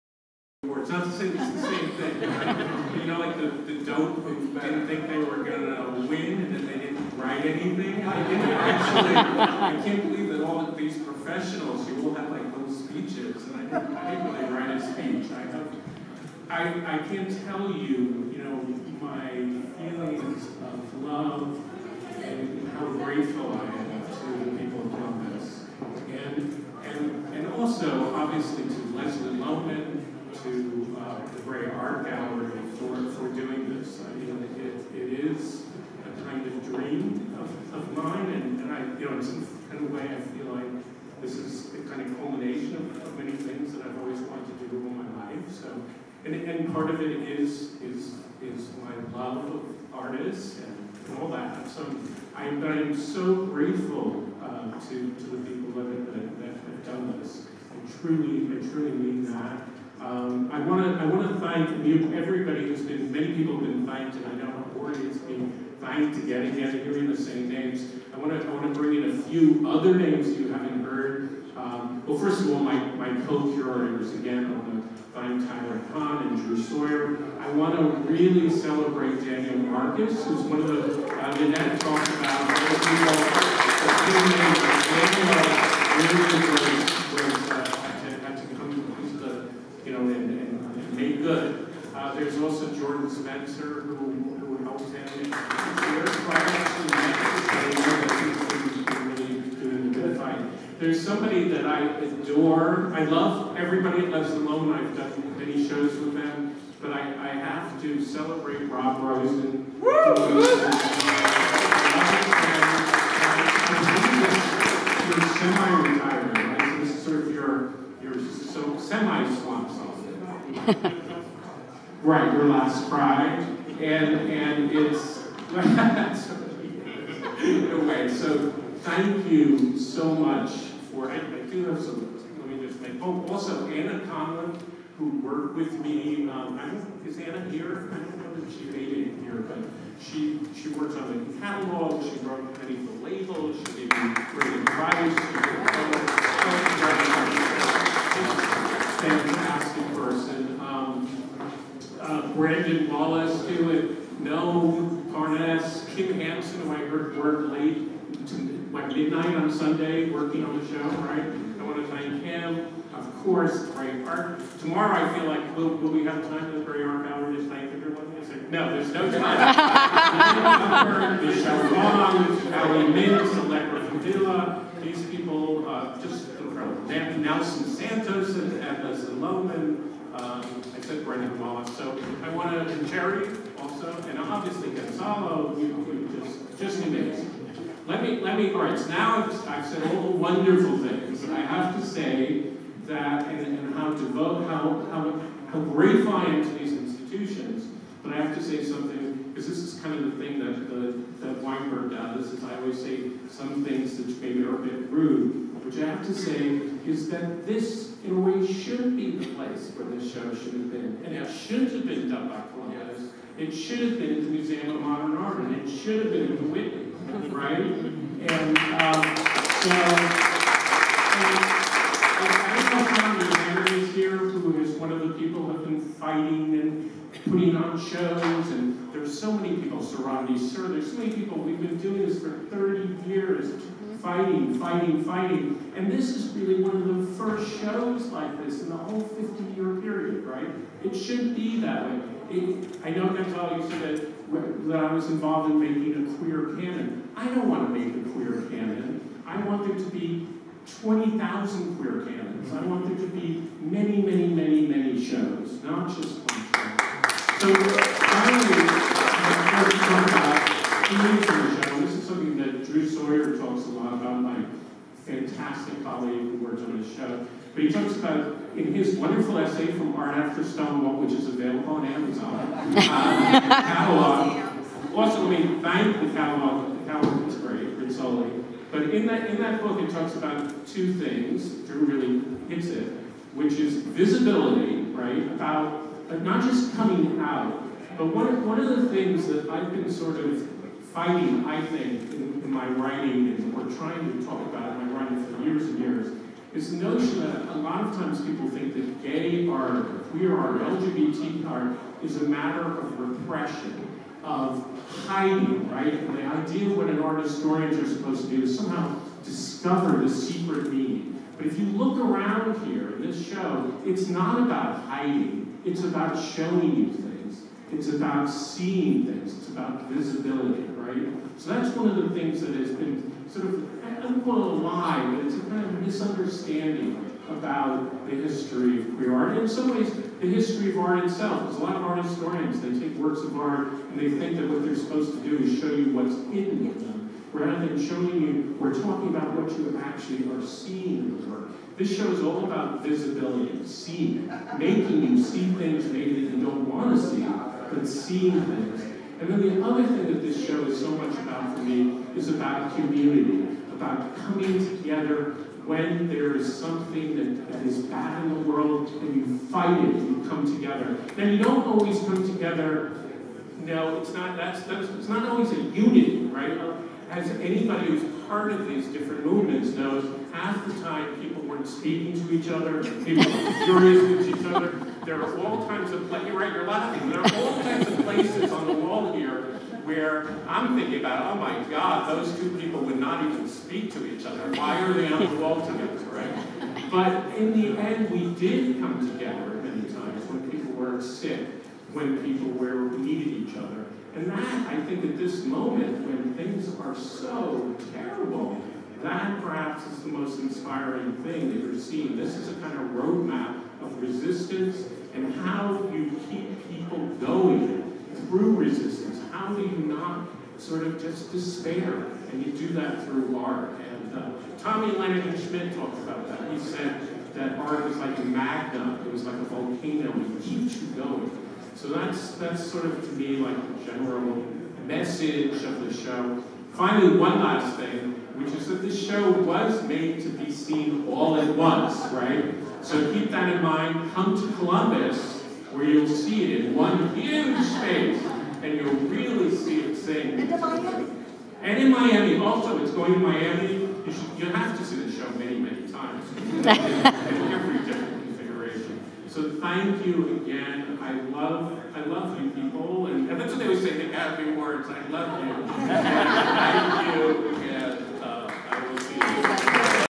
Art After Stonewall Preview at Museum of Gay and Lesbian Art. April 23, 2019.
Remarks from Jonathan Weinberg at the preview reception for "Art After Stonewall: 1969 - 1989."

Clip BDMV 4.